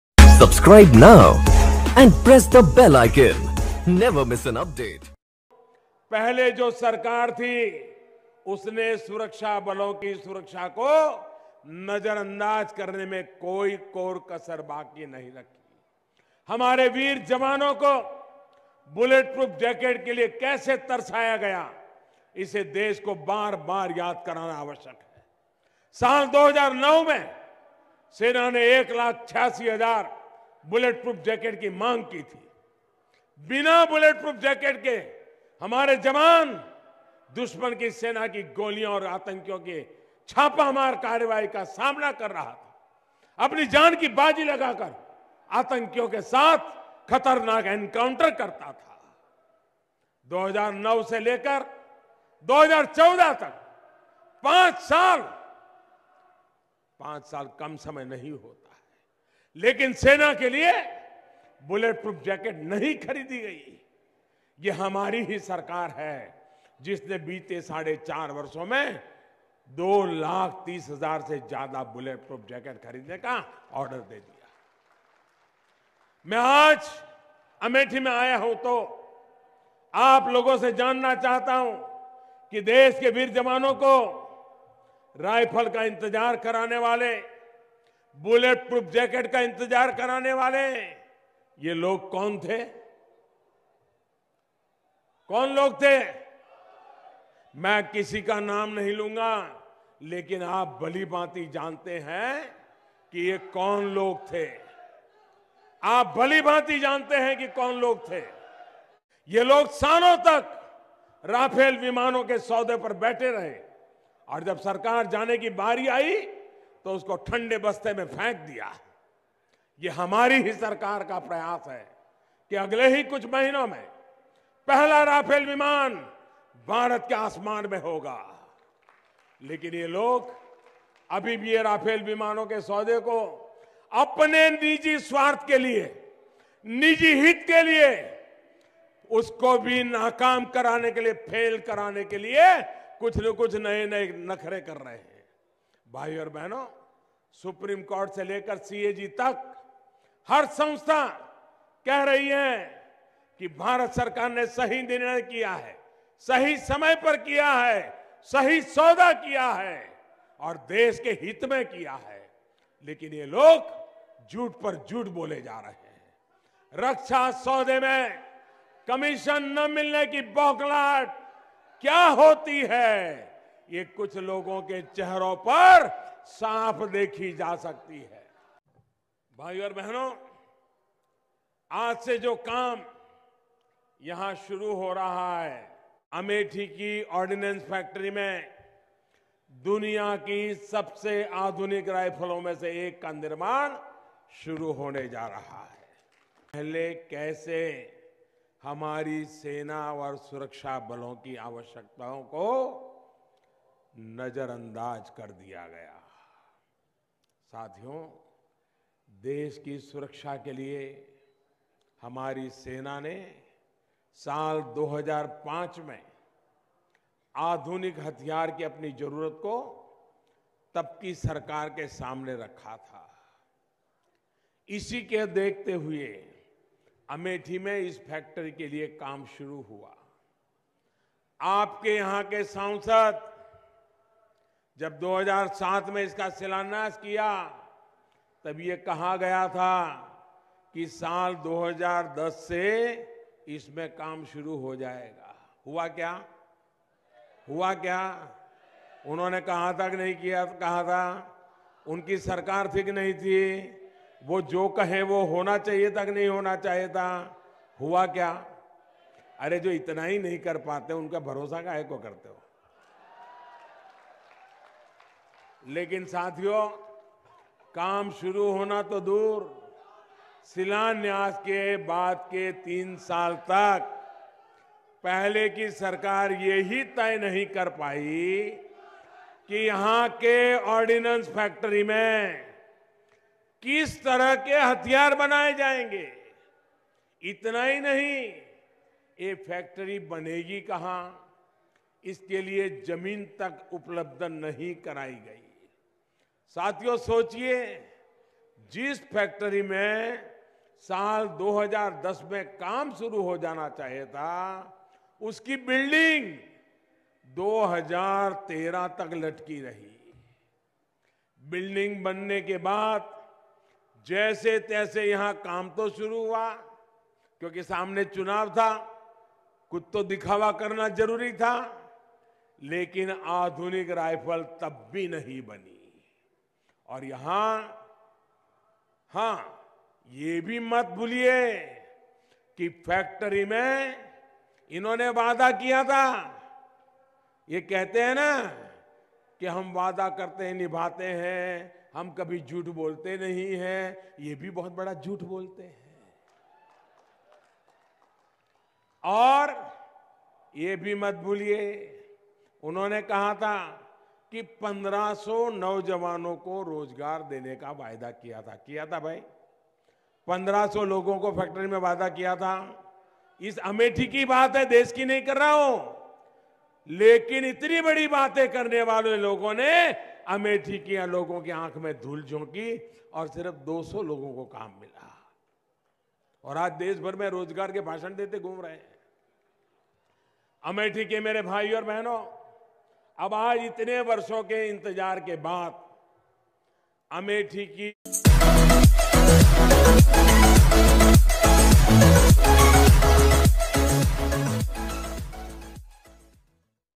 PM Modi ने बताया किस तरह सेना की security को लेकर खिलवाड किया है Congress ने ।
PM Modi attacks UPA Govt, says failed to fulfil the basic necessity of the armed forces even after several decades in power. Modi says, "In our tenure, the corridors of power were free of middlemen because they know this government will not tolerate corruption. We managed to obtain 2,30,000 bulletproof jackets for our armed forces," Watch video,

पीएम मोदी ने सेना की सुरक्षा से खिलवाड़ करने पर कांग्रेस पर जमकर हमला बोला. पीएम मोदी ने कहा कि 2009 में हमारी सेना ने 1 लाख बुलेट प्रूफ जैकेट की मांग की. लेकिन 2009 से 2014 तक एक भी बुलेट प्रूफ जैकेट नहीं खरीदी गई. हमने अपने कार्यकाल में 2.30 लाख बुलेट प्रूफ जैकेट खरीदीं, और ऐसे लोग हमसे सवाल पूछते हैं.

CREDIT-BJP.